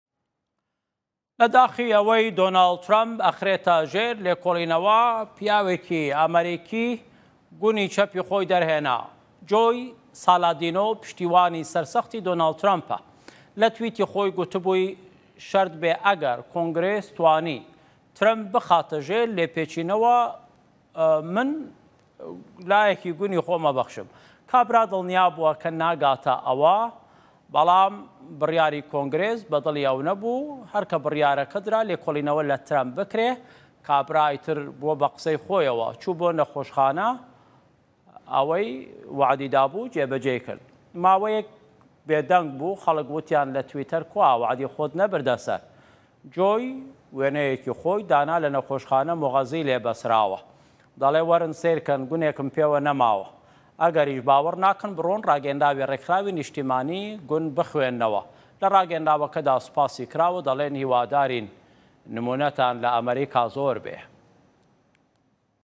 شاهۆ ئەمین: پیاوێکی ئەمریکی لەسەر لێپێچینەوە لە ترەمپ گونێکی لەدەستدەدات
شاهۆ ئەمین: پیاوەکە گوتی شەردبێ ئەگەر کۆنگرێس توانی ترەمپ بخاتە ژێر لێپێچینەوە من لایەکی گونی خۆم دەبەخشم